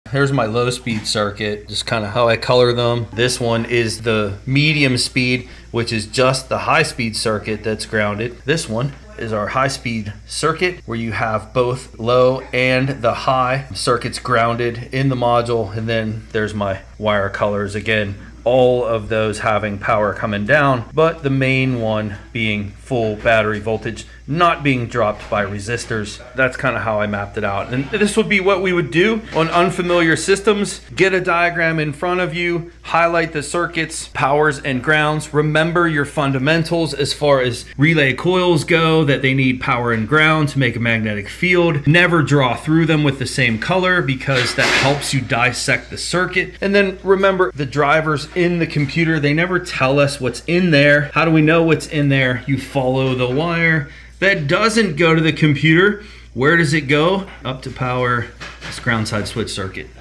Dissecting a wiring diagram [2017 Chevy Trax Cooling Fan Circuit]
[2017 Chevy Trax Cooling Fan Circuit]
 Fan was running continuously and killing the battery. Full video and lesson is on my website.